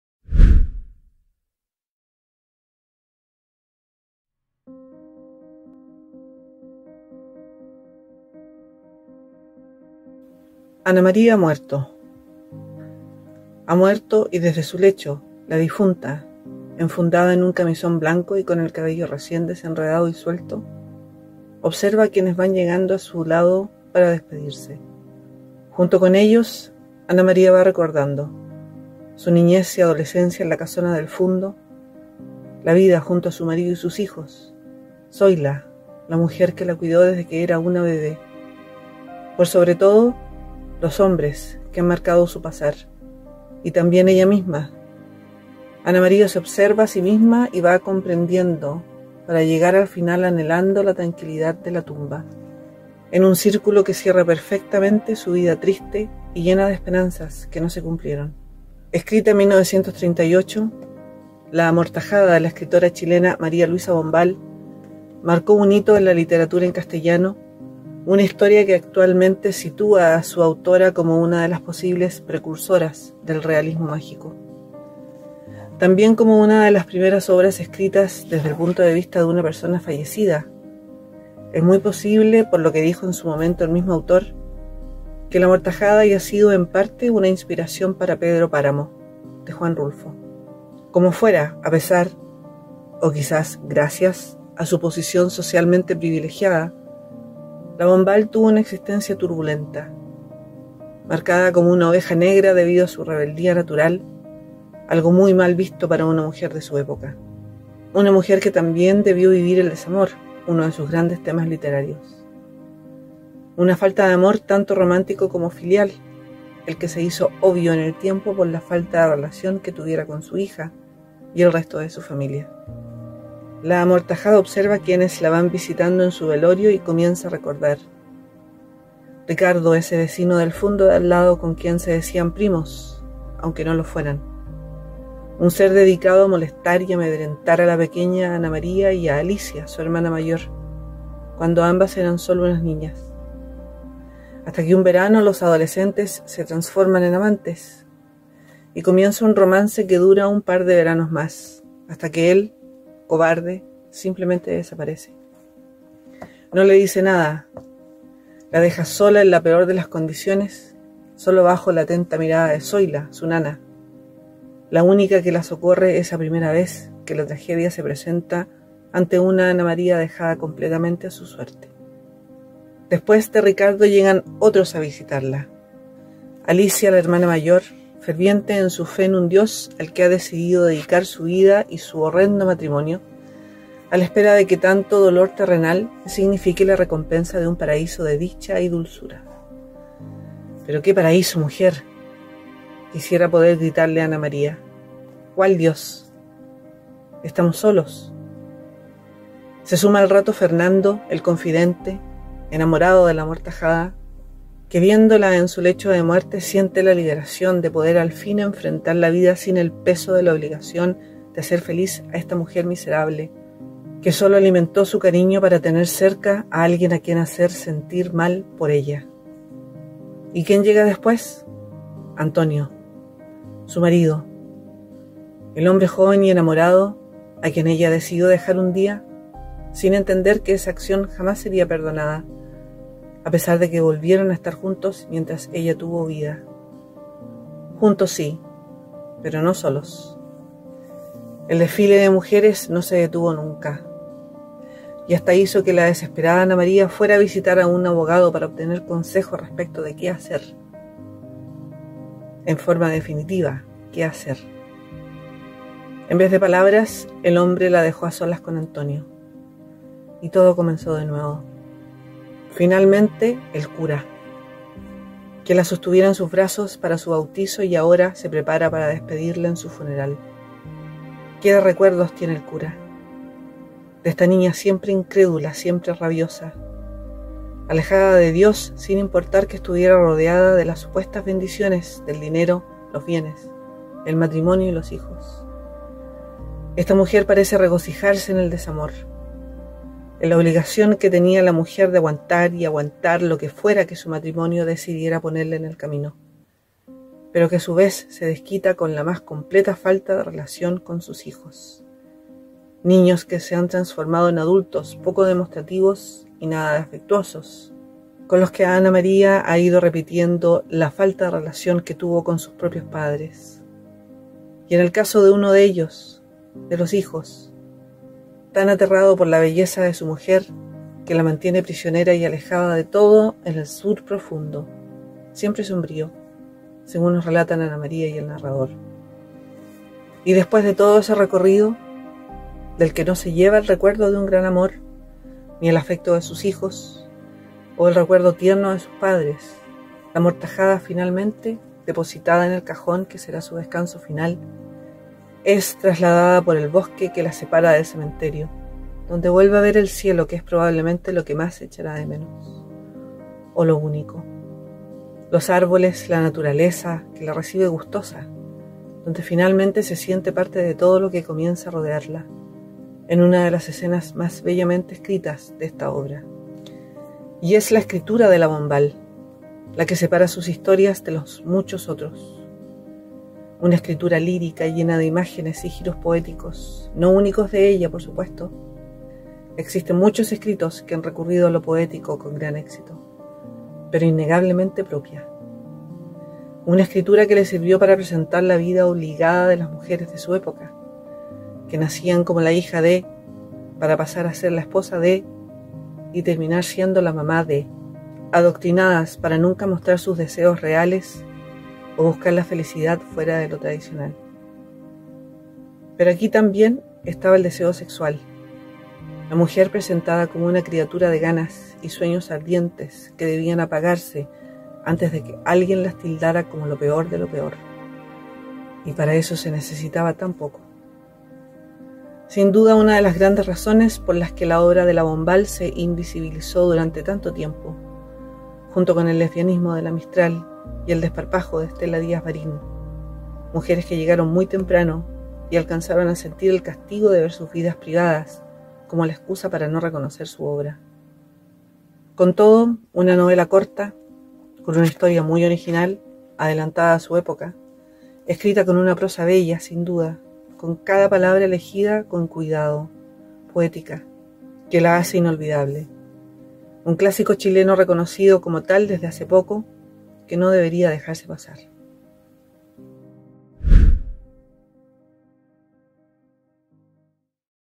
La amortajada, María Luisa Bombal
Desde su lecho de muerte, Ana María observa a quienes llegan a velarla y recuerda su vida: la falta de amor romántico, o del amor romántico como ella lo hubiera deseado. Ideal. La vida miserable de su hermana, Alicia, tan entregada a un pasar horrible como esposa y madre con la esperanza de la vida eterna en un paraíso dulce y tranquilo. Fernando y el amor que la amortajada jamás correspondió pero sí aprovechó. Su primer amor, y su primer desengaño. Y su marido, perdido desde siempre ante las otras mujeres. Los hijos que no la echarán de menos.
Una vida completa anhelando el amor que Ana María jamás fue capaz de entregar, con una prosa poética inimitable y un estilo de escritura solo de la Bombal. Un clásico chileno que no se debe perder.